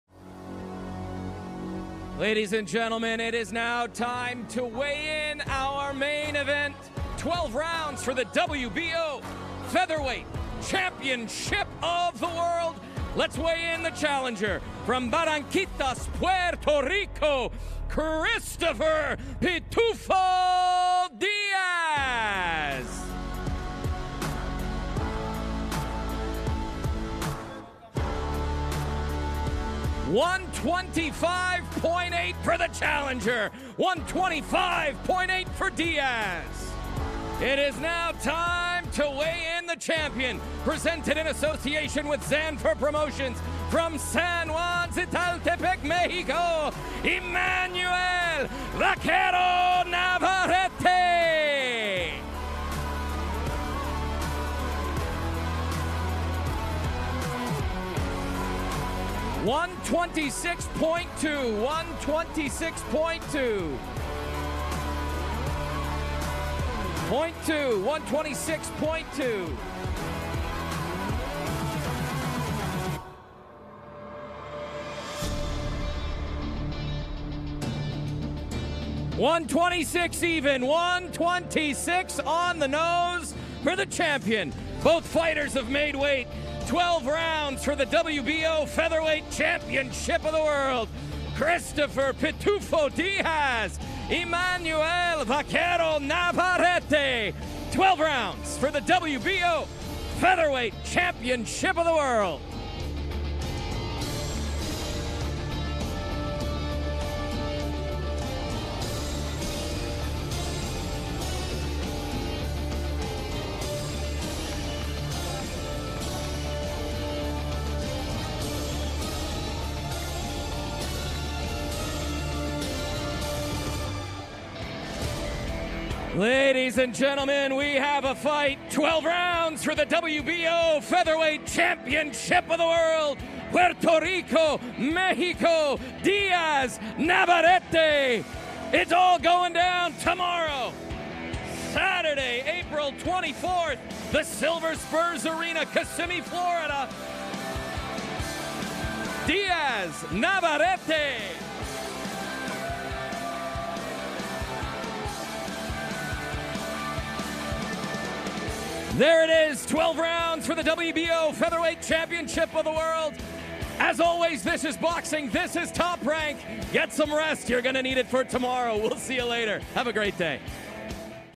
Emanuel Navarrete & Christopher Diaz Make Weight, Title Fight Official Tomorrow on ESPN & ESPN+
Watch highlights from the main event weigh in of Navarrete vs Diaz Featuring EmanuelNavarrete and ChristopherDiaz. Both Fighters made weight and now the title fight is official for tomorrow.

Next up for Top Rank Boxing, Mexican Star Emanuel Navarrete to Defend Featherweight World Title Against Puerto Rican Contender Christopher Diaz LIVE on ESPN, ESPN Deportes and ESPN+

Knockout sensation Edgar Berlanga (16-0, 16 first-round KOs) to battle Demond Nicholson in super middleweight co-feature.

NavarreteDiaz​ and Berlanga vs Nicholson​ can been seen on ESPN, ESPN Deportes, and ESPN+ from Kissimmee, Fla. at 10 p.m. ET/7 p.m. PT. The undercard, including top Puerto Rican prospects Josue "The Prodigy" Vargas, Joseph "Blessed Hands" Adorno and Xander Zayas, will stream live on ESPN+ at 6 p.m. ET/3 p.m. PT.